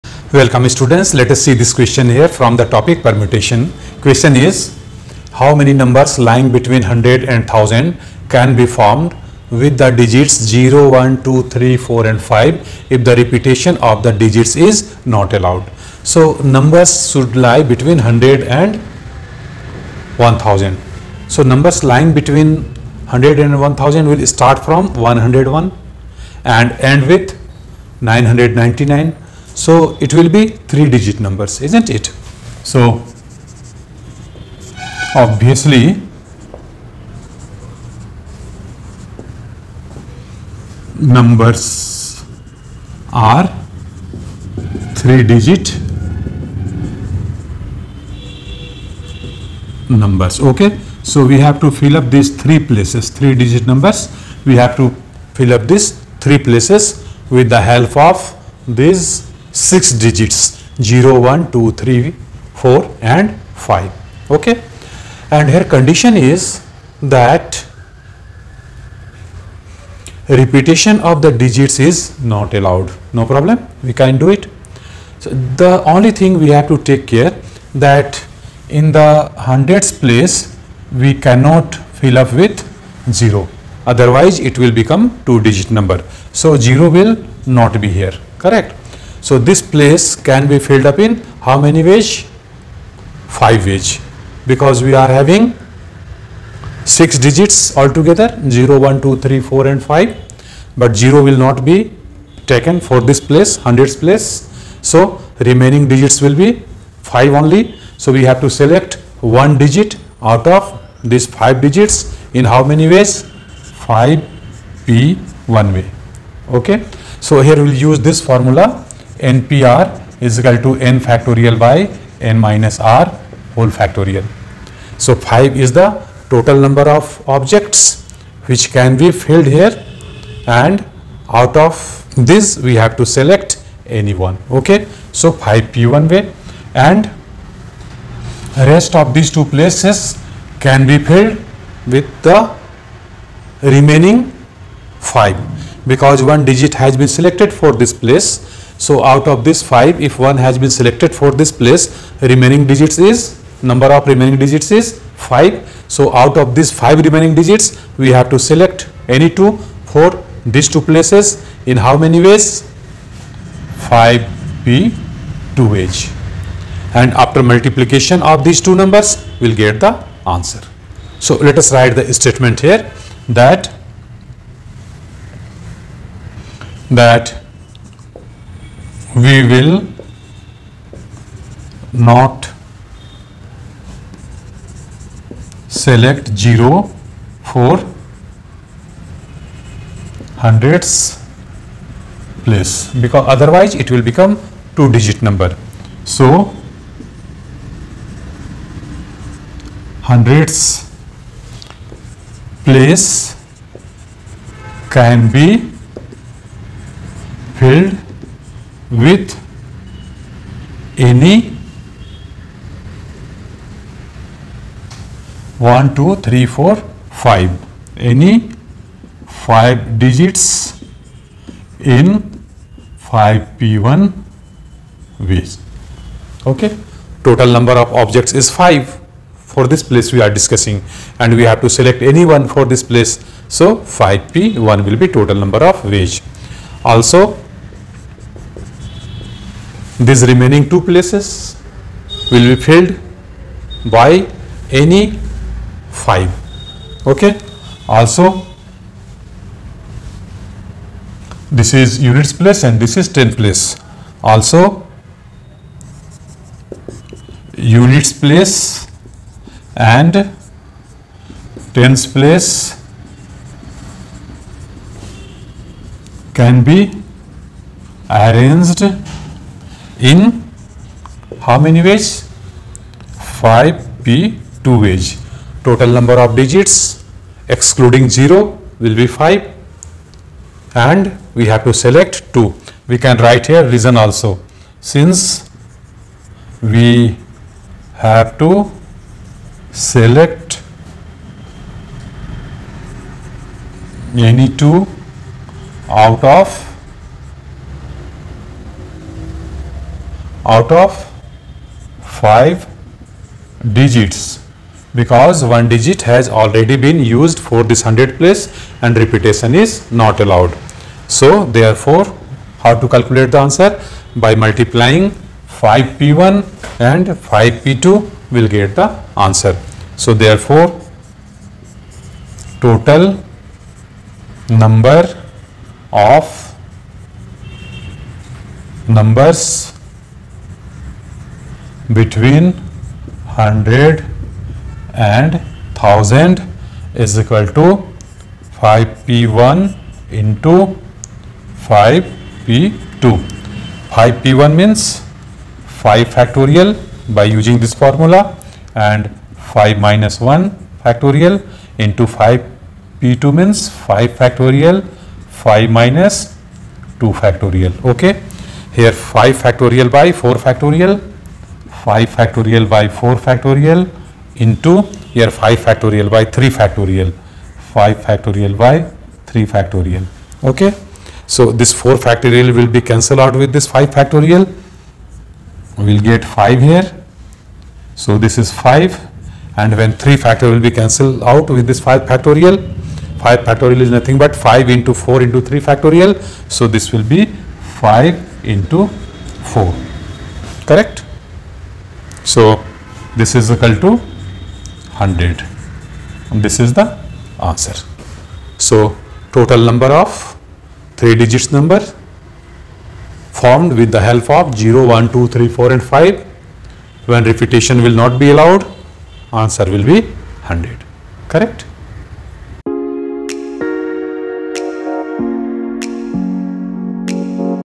How many numbers lying between 100 and 1000 can be formed with the digits 0,1,2,3,4 and 5
How many numbers lying between 100 and 1000 can be formed with the digits 0,1,2,3,4 and 5, if the repetition of the digits is not allowed